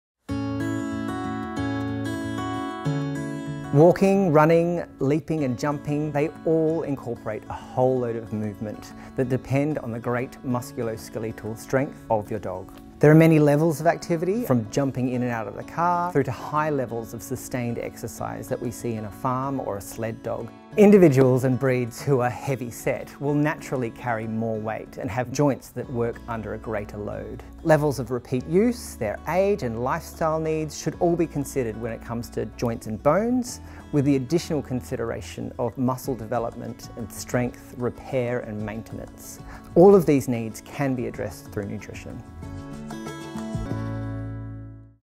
Why is joint & muscle health so important for dogs
In this video Dr Lee Danks, Veterinarian at Black Hawk, gives his tips on how to identify if your dog may need joint & muscle support.
Spending time walking, running, leaping, jumping, pawing, playing, bowing, scratching, greeting and digging is what life is all about, and a great marker of health and wellbeing. Just as it is with us humans, it’s only when we have a fall or sprain or injury or even disease process which affects our joints and muscles that we truly appreciate how what’s called our ‘musculo-skeletal systems’ (combined muscles, bones and joints altogether) are central to a good quality of life.